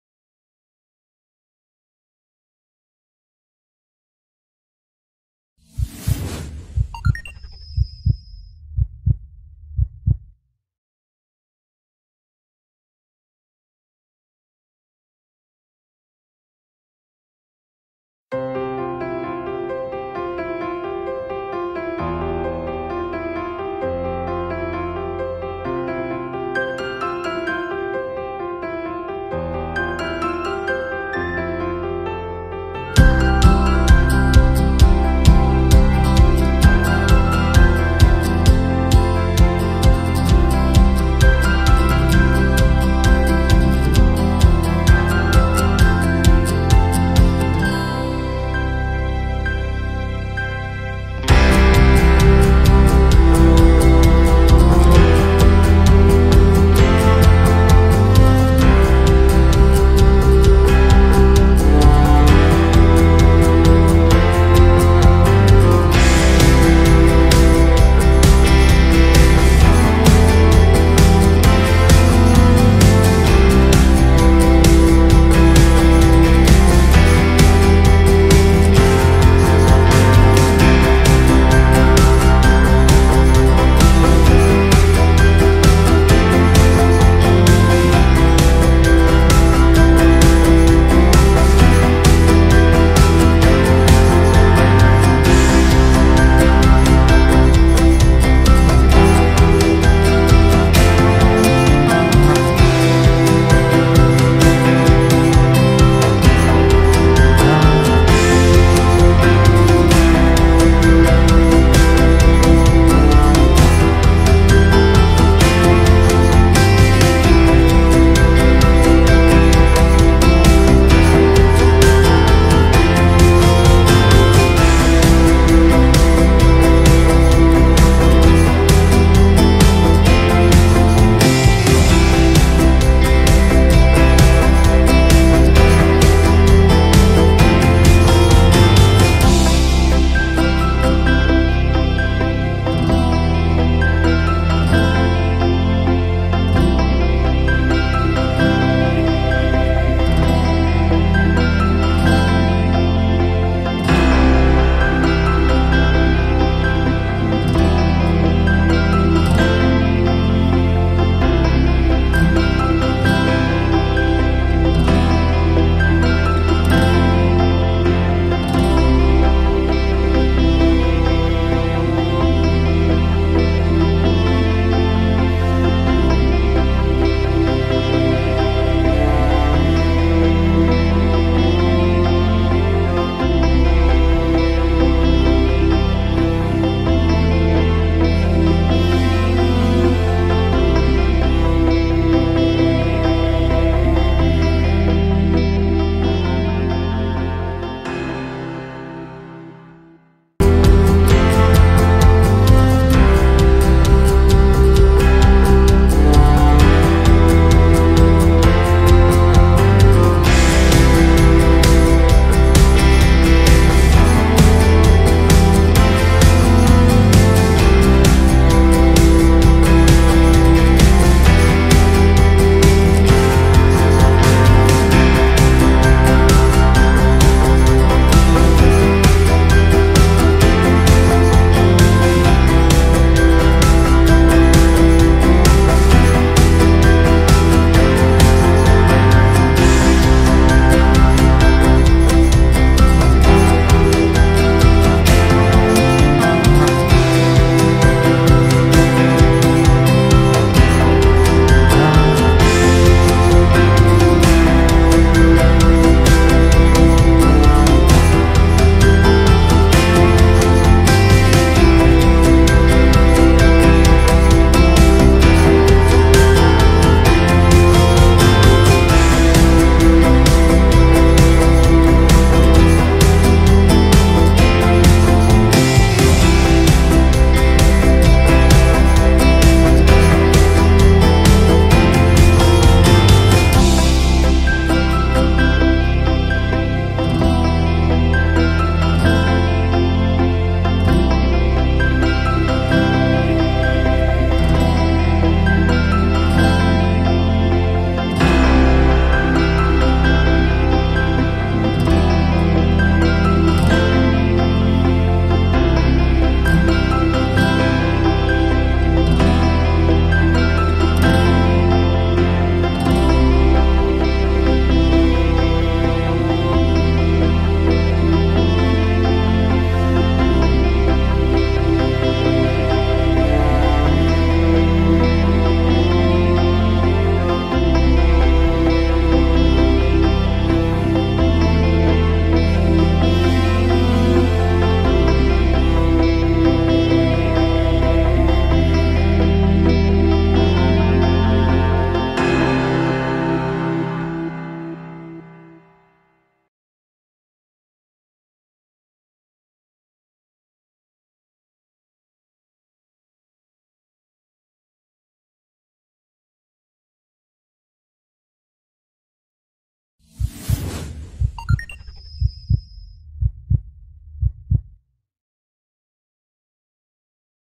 Tips para tu Acuario | Conoce Fondo de Bikini
Sabías que contamos con un acurio llamdo Fondo de Bikini, conoce un poco más sobre este.